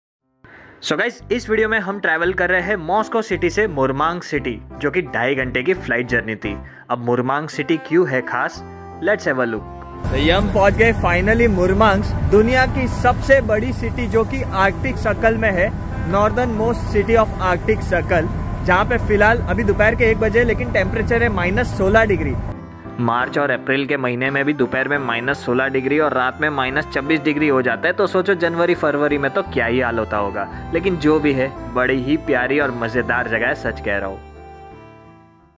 Finally in Arctic circle - Murmansk, Russia 🇷🇺
Arctic circle ki biggest city pohoch gaye hum.
-24° C temperature hai.
Full video on - pratik jain vlogs